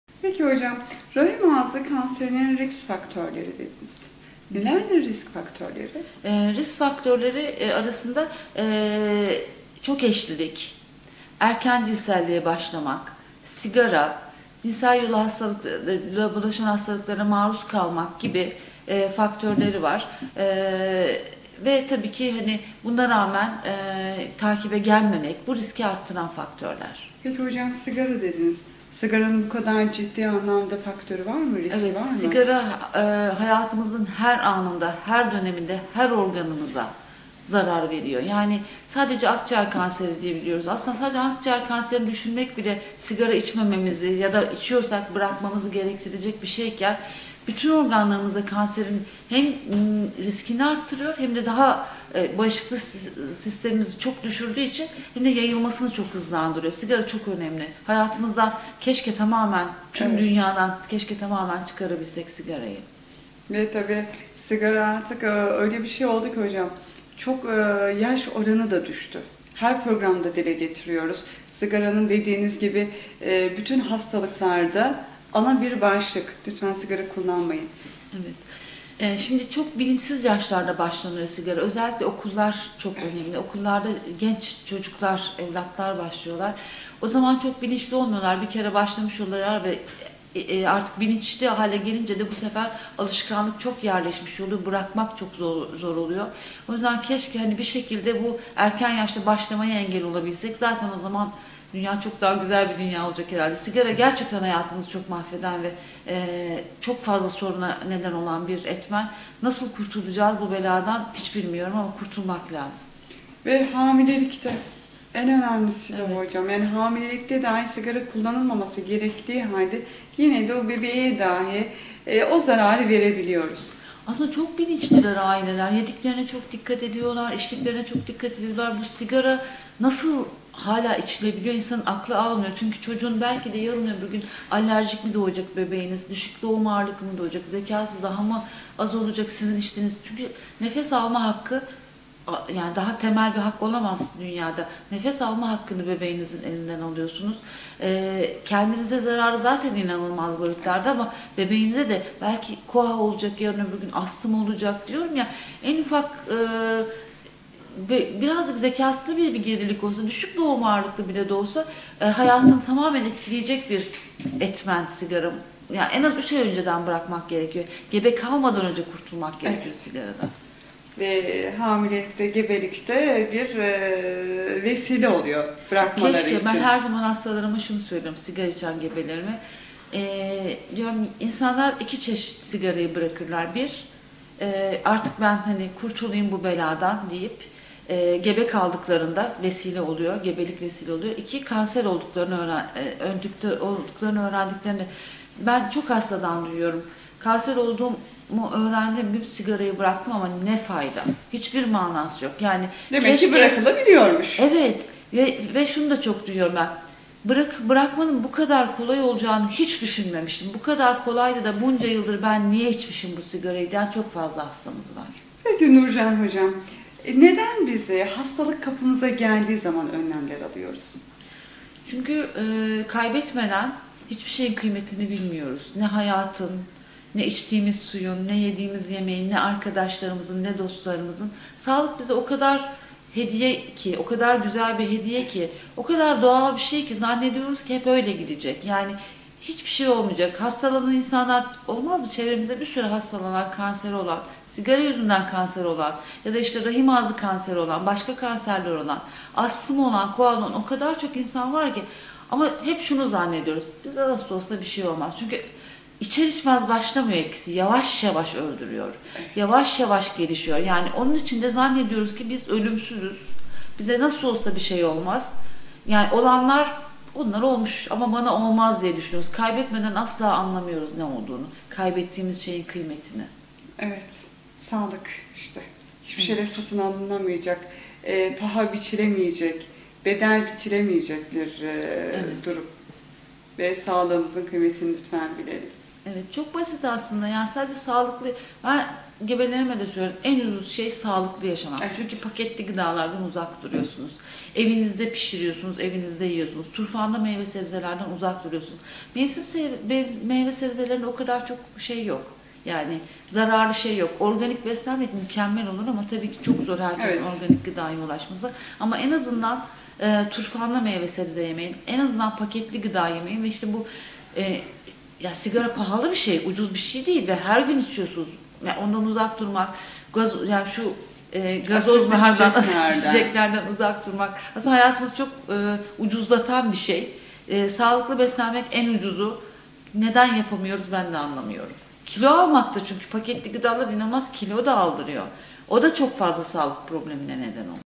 Rahim Ağzı Kanserinin Risk Faktörleri Nelerdir ?
Avrasya Hastanesi Kadın Hastalıkları Ve Doğum Uzmanı Op. Dr. Nurcan Dalan '' Rahim Ağzı Kanserinin Risk Faktörleri Nelerdir ? '' hakkında detaylı bilgiler veriyor.